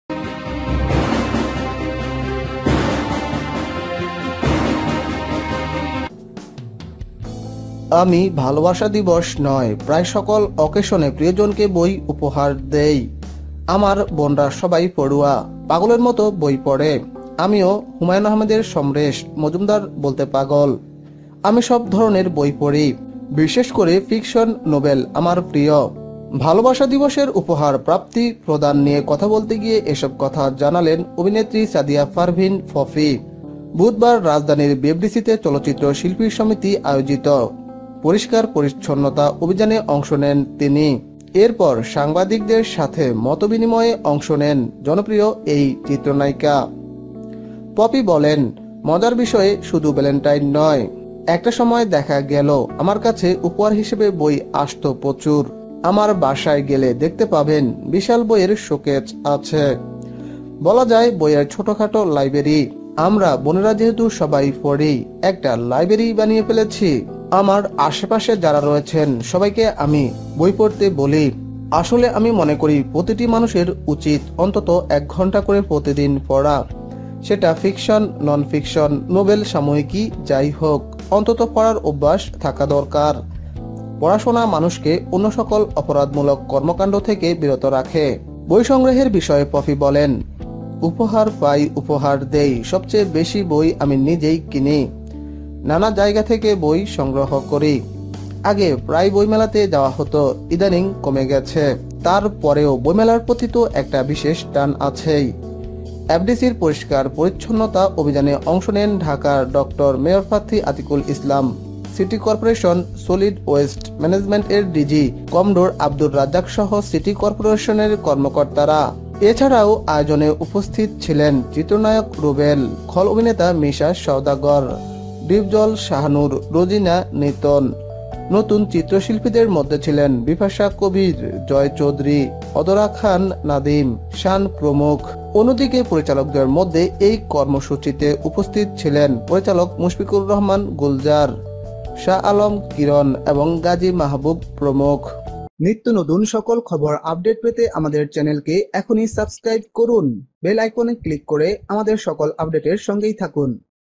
প্রকাশ্যে প্রিয় মানুষের বিষয়ে মুখ খুল্লেন পপি !! কি উপহার দিলেন তাও জানালেন !! Bangla news
❥ 0:10